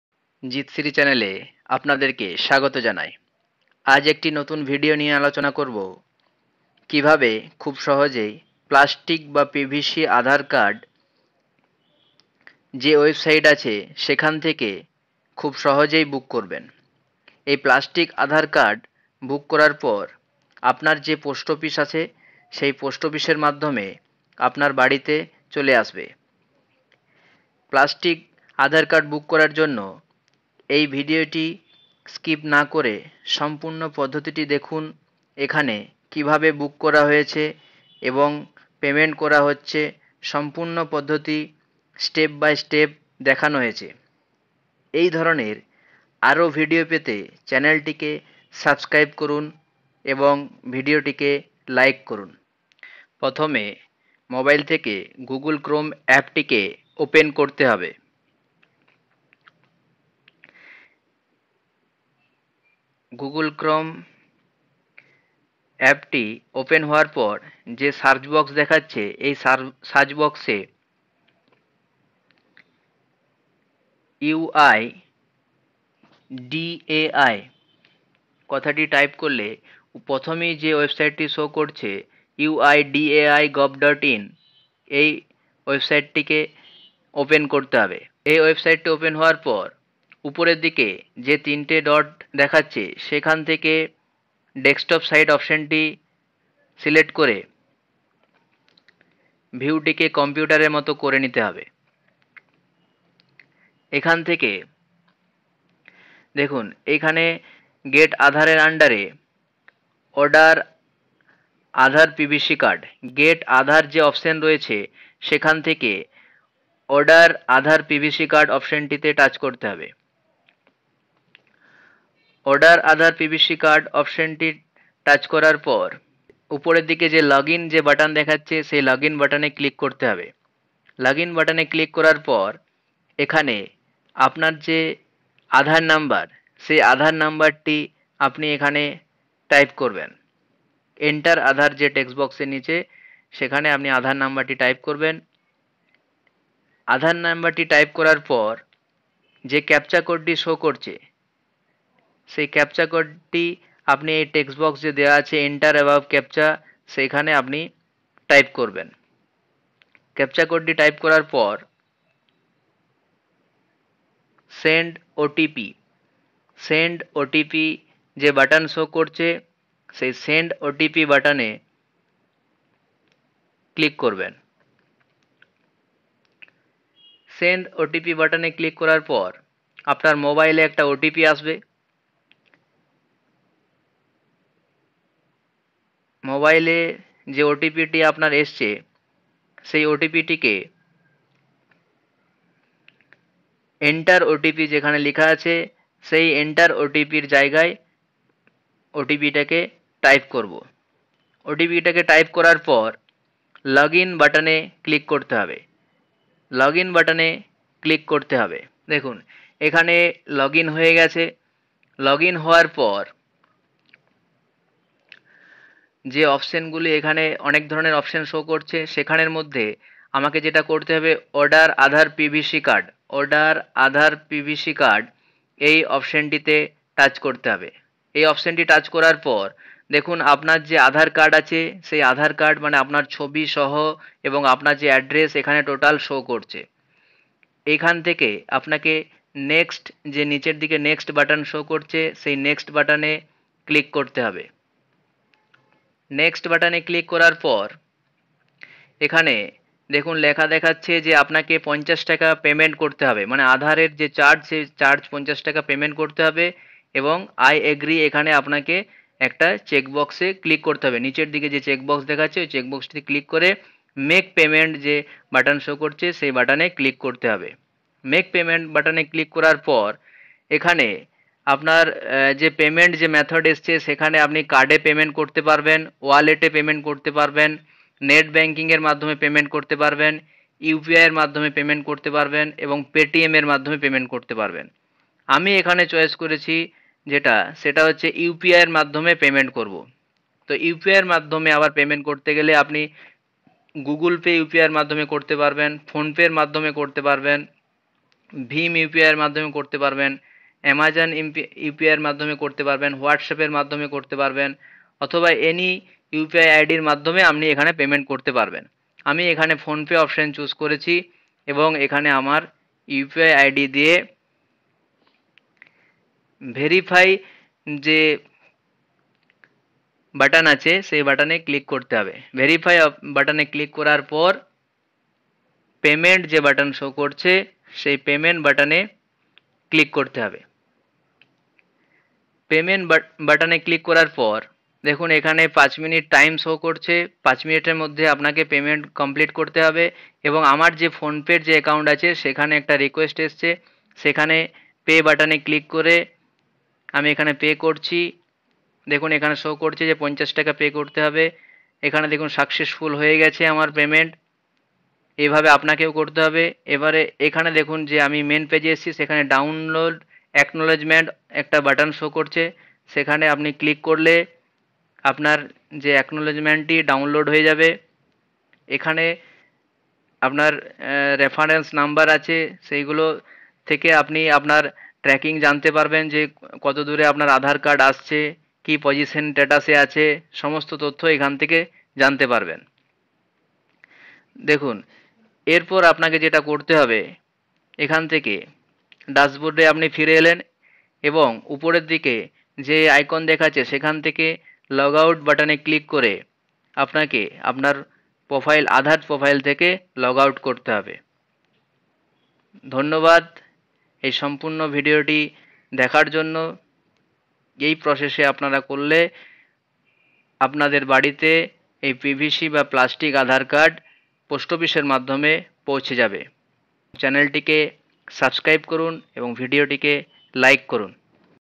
How to Order PVC Aadhar Card in Mobile || PVC Aadhar Card Kivabe Pabo
This video discussed how to order pvc aadhar card in mobile. Step by step Online apply process and payment.
pvc aadhar card order
aadhar card pvc online apply